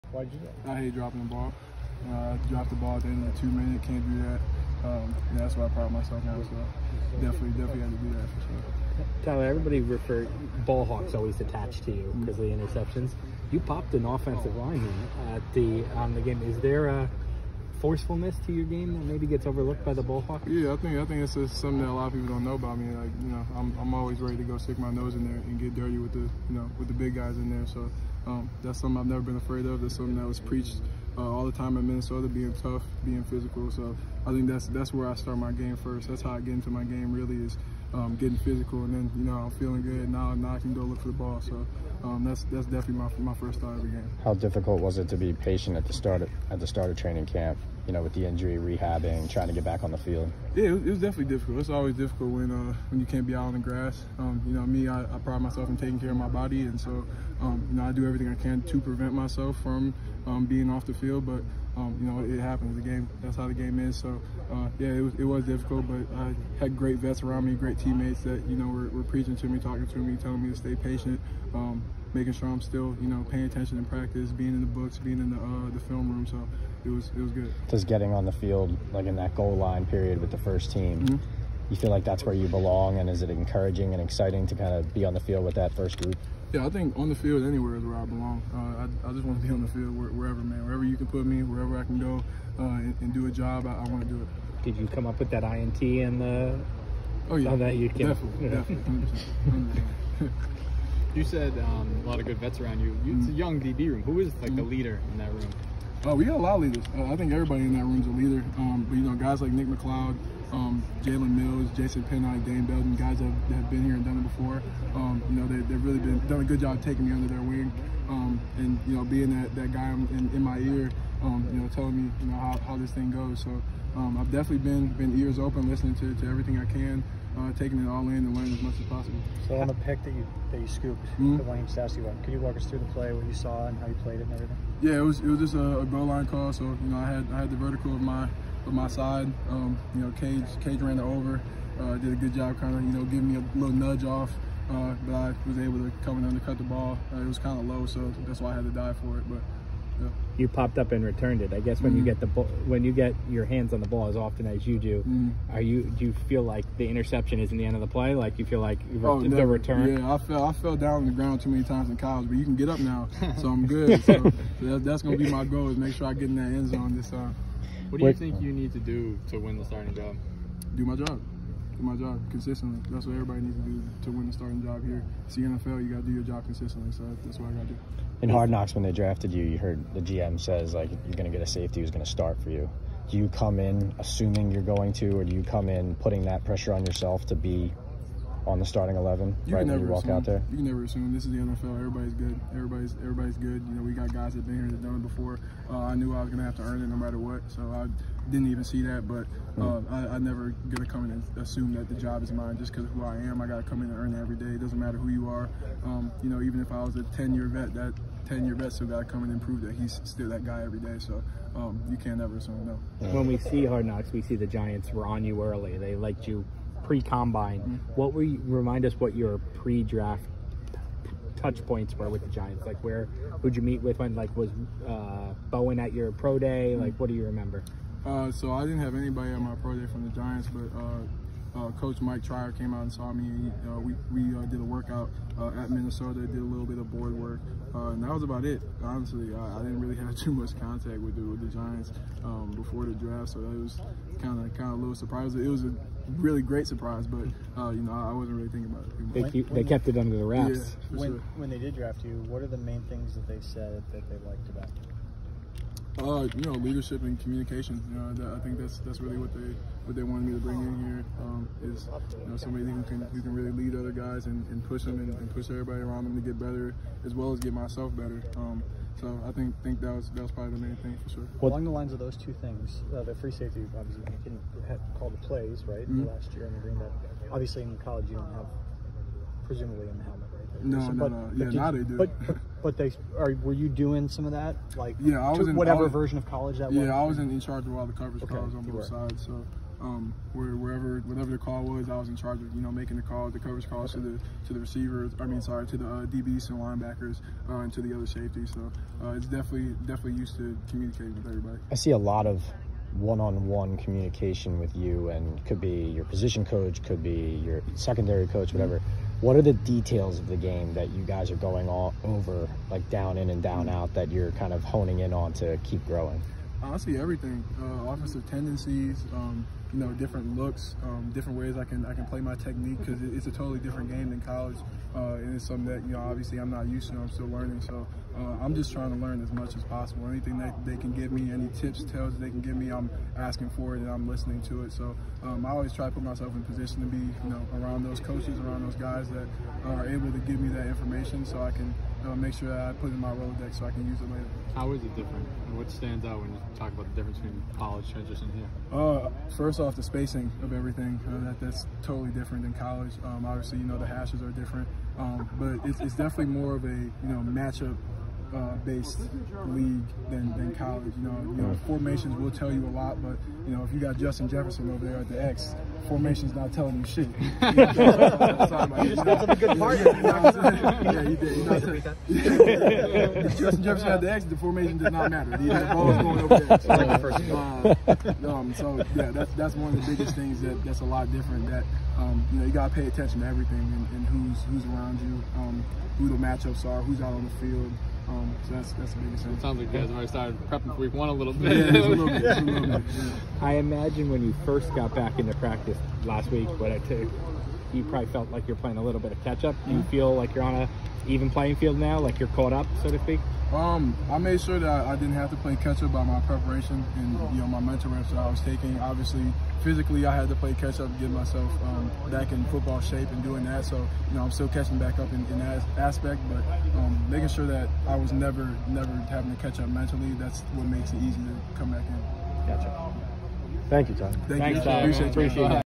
Giants rookie S Tyler Nubin on ballhawking, first-team reps, NFL adjustment & a draft night surprise
Giants rookie safety Tyler Nubin, a second-round pick from Minnesota, talks about his interception of Daniel Jones in practice, his push-ups after a drop, first-team reps, a crushing hit on a Detroit Lions lineman, the NFL adjustment, a draft night surprise and more in this interview on Aug. 12, 2024.